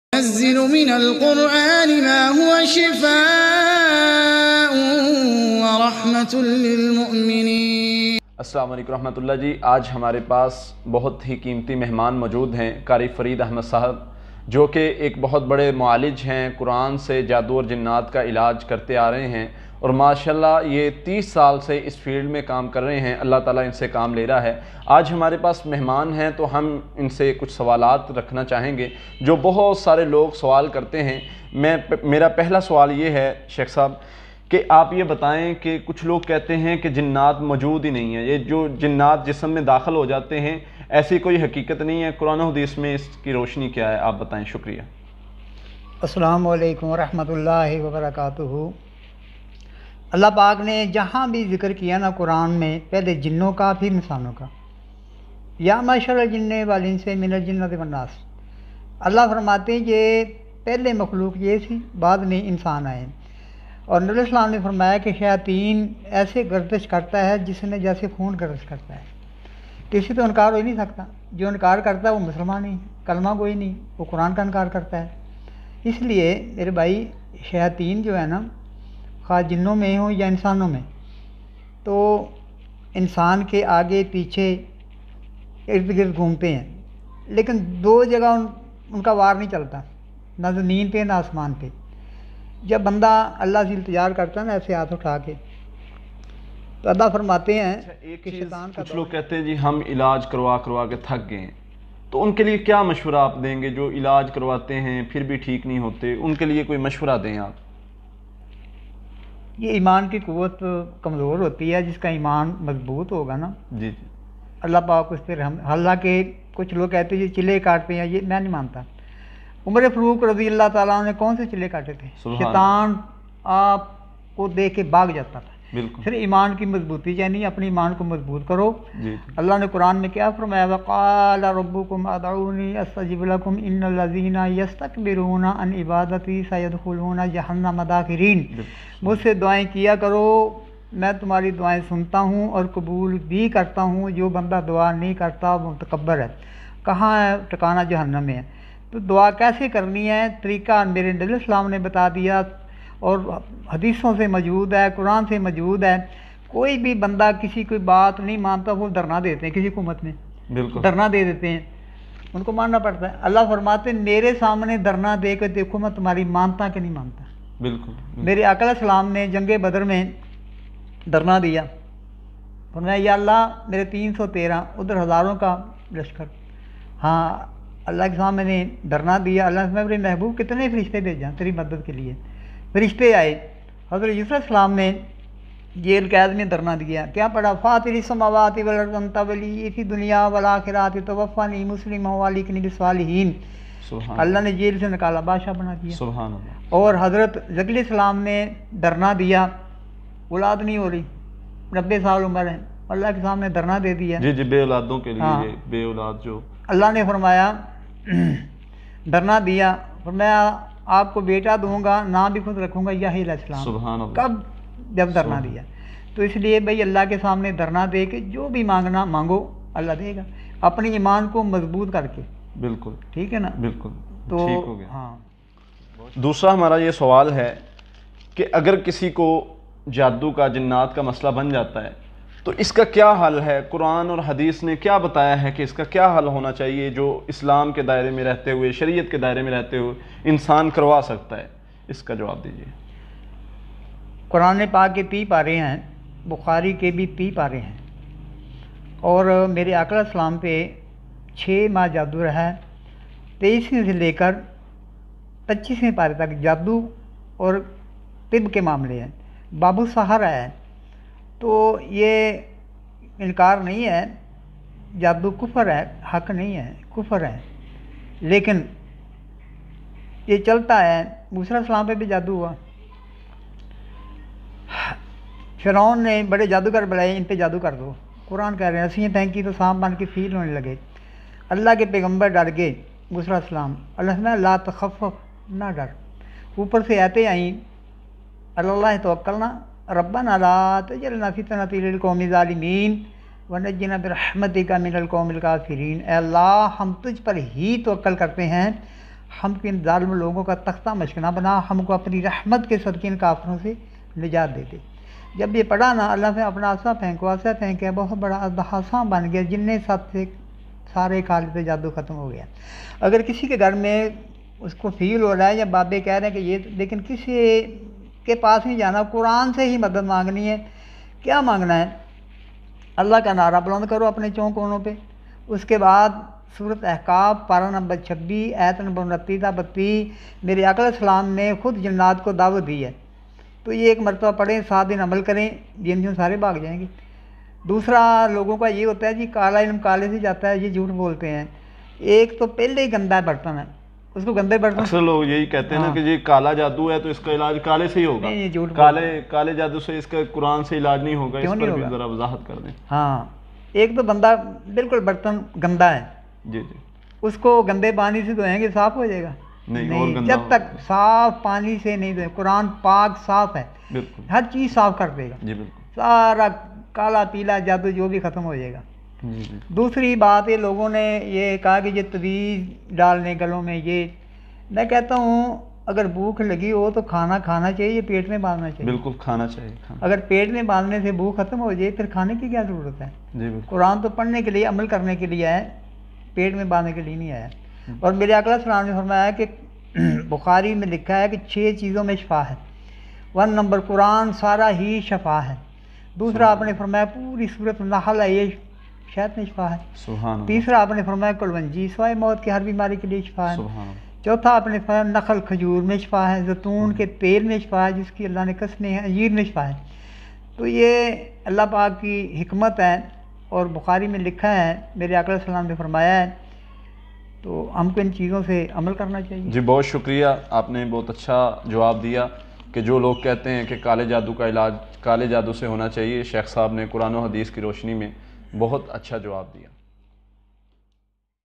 Qari Fareed Ahmad sy Jadu Jinnat Ky baray ma Sawalat with HAfiz Abdul Basit Salfi
Qari Farid Ahmad sy Jadu Jinnat Ky baray ma Sawalat with HAfiz Abdul Basit Salfi

This Topic is Related By

     Hafiz ABdul Basit Salfi

Note:

IF I m Busy drop your MSG on my cell number. )